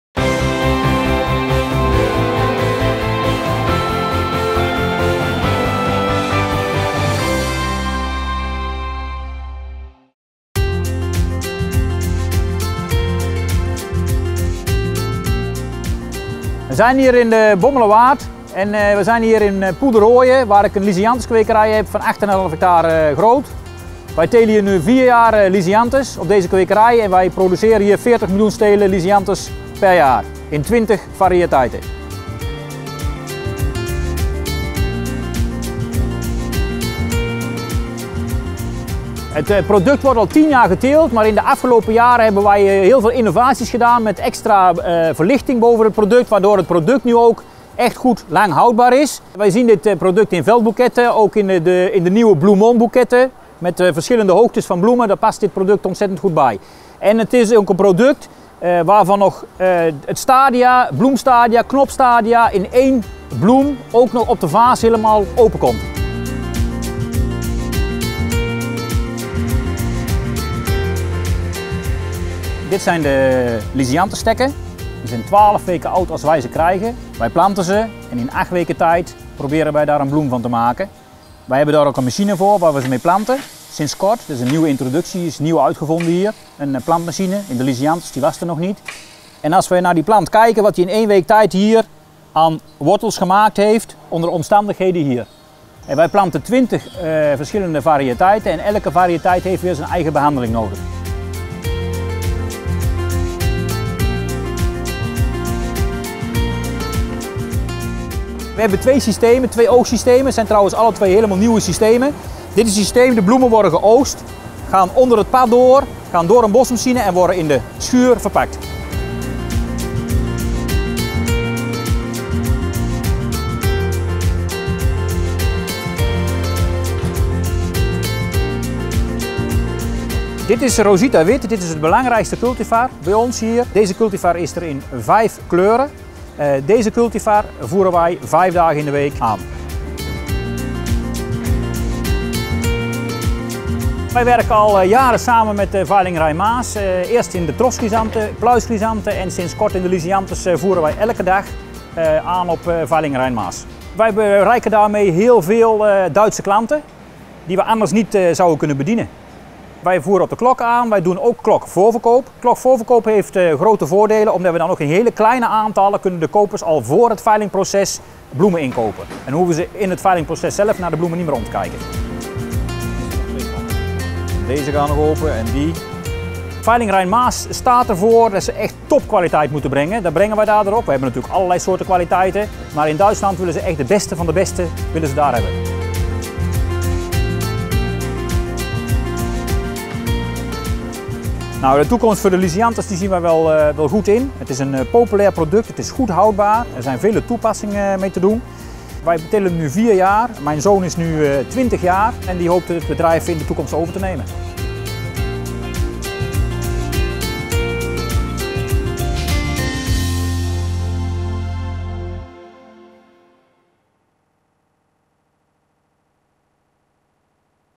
Kwekerij Waalzicht im Rampenlicht
„Wir produzieren 20 Sorten Lisianthus in unserer Gärtnerei in der Region Bommelerwaard. Unser Ziel ist es, immer Blumen in Spitzenqualität anzuliefern. Die Kunden von Veiling Rhein-Maas kaufen am liebsten `das Beste vom Besten´ bei der Versteigerung “, erläutert Jan van Wijk, der den Betrieb leitet. Im neuen „Anlieferer im Rampenlicht“-Video stellen wir Ihnen die „Kwekerij Waalzicht“ aus Poederoijen vor.

Auf einer Fläche von 8,5 Hektar produziert der Betrieb 40 Millionen Lisianthus-Stiele pro Jahr. Die Rosita ist in fünf Farben erhältlich, am wichtigsten ist aber Rosita White. Die „Premium Collection“ der „Kwekerij Waalzicht“ erkennt man an der Schwere und Blütenmenge je Zweig.
Für Jan van Wijk ist die Versteigerung noch stets der wichtigste Verkaufskanal. Der Betrieb liefert an allen Versteigerungstagen seine Produkte bei der Versteigerung in Straelen-Herongen an. Darüber hinaus wird ein Teil der Produkte über den Klokservice verkauft und auch der Verkauf über den digitalen Verkaufskanal Uhrvorverkauf nimmt stark zu. Innovation ist für den Betrieb sehr wichtig, so wurde bereits die Beleuchtung des Gewächshauses optimiert, was eine längere Haltbarkeit der Lisianthus zur Folge hat. Außerdem sind zwei neue Erntesysteme in Betrieb – für das Einpflanzen wurde speziell für den Betrieb eine neue Maschine entwickelt. Hierbei handelt es sich um eine neue Erfindung.